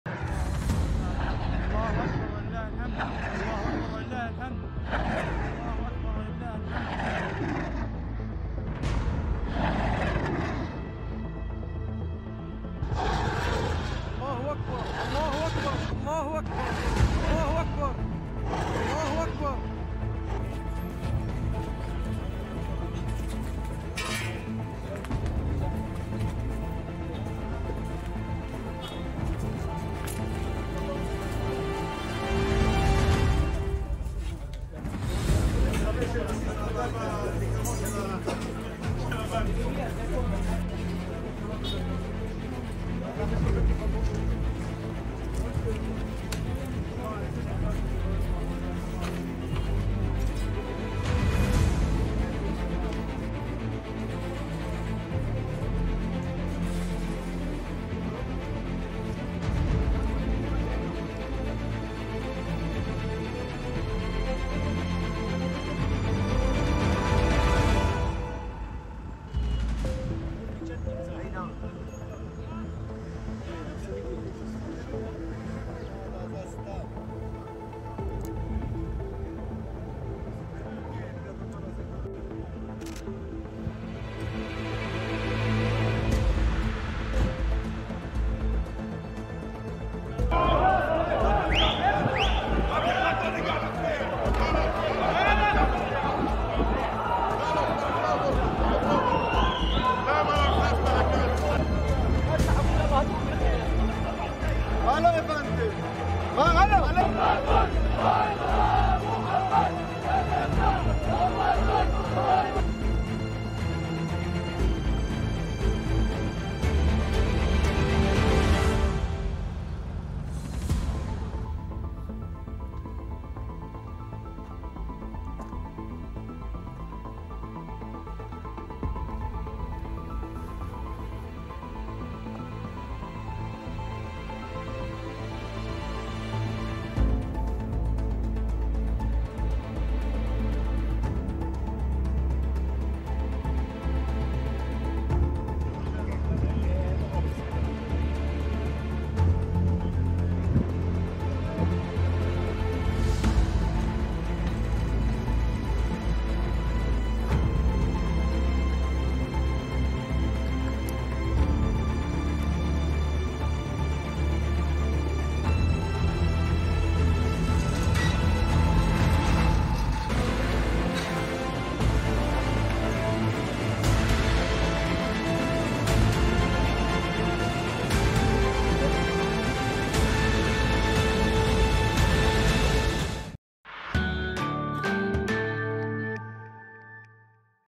إطلاق صواريخ ورد بالمدفعية.. ماذا يحدث على حدود لبنان وإسرائيل؟
أعلنت إسرائيل أنّ أكثر من 30 صاروخاً أُطلقت عصر الخميس من جنوب لبنان باتّجاه أراضيها الشمالية، في قصف أوقع جريحاً وأضراراً مادّية. وقالت إسرائيل إنّها “نيران فلسطينية” وليست هجوماً مباشراً من حزب الله.